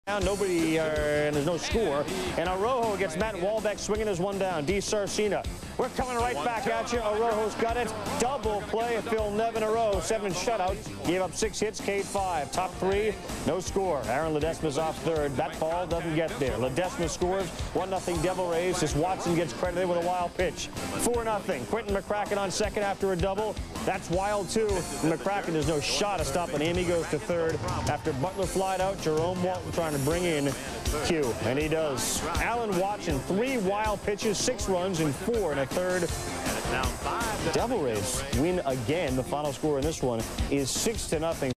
Anaheim Angels vs Tampa Bay Devil Rays (highlights) - April 19, 1998
Featuring Rolando Arrojo, Aaron Ledesma, and Jerome Walton